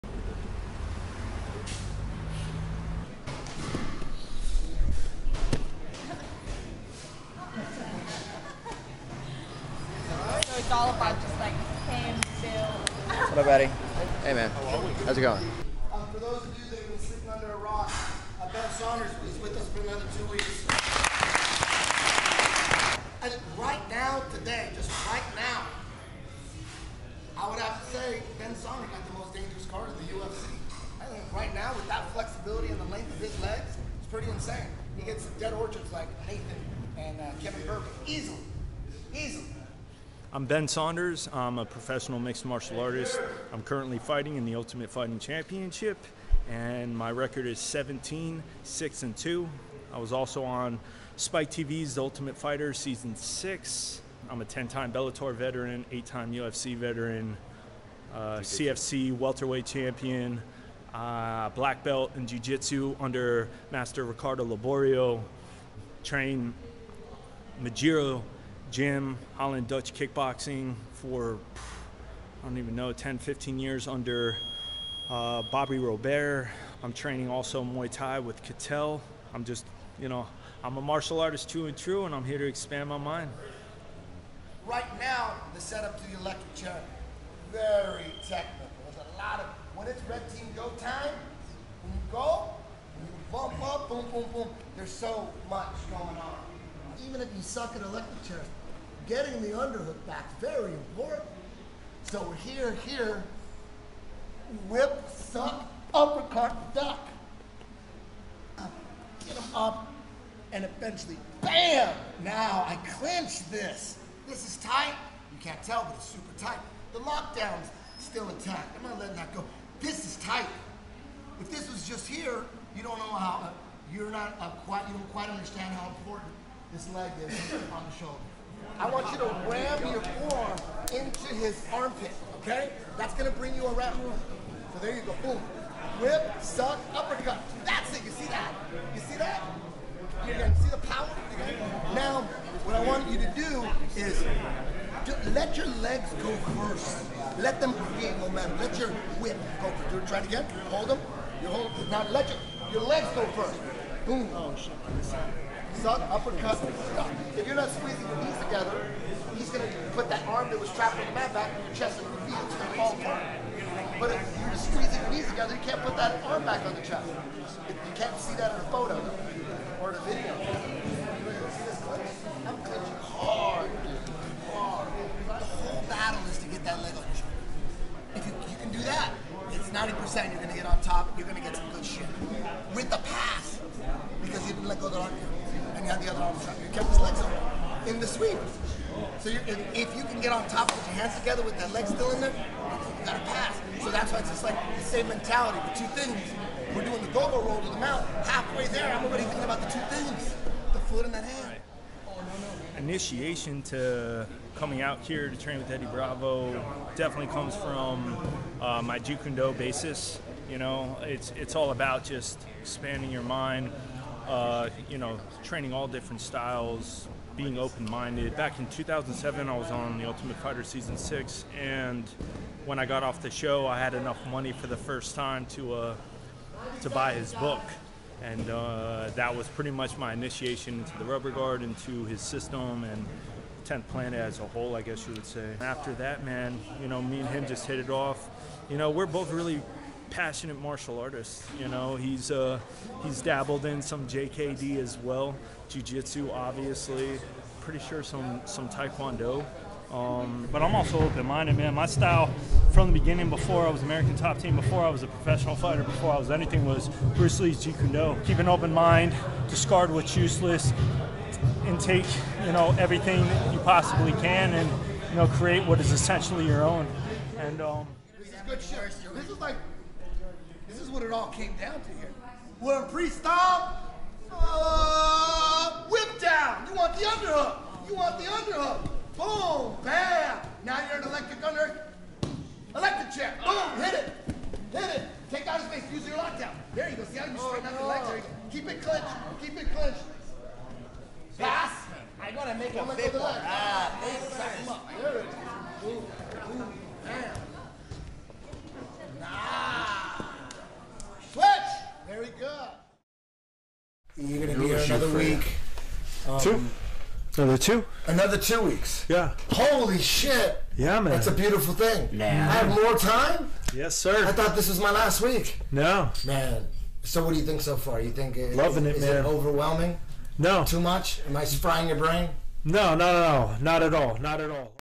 UFC fighter Ben Saunders’ Training camp at 10th Planet HQ - Part 1
Ben Saunders spent part of his fight camp at 10th Planet HQ in downtown LA. In preparation for UFC ON FOX 13 (December 13, 2014) he trains an unorthodox style of jiujitsu with an open mind akin to the Legendary Bruce Lee.

0:16 - Introducing Ben Saunders
1:25 - Learning Electric Chair
3:20 - Initiation to 10th Planet and Jeet Kune Do
4:25 - Electric Chair Mastery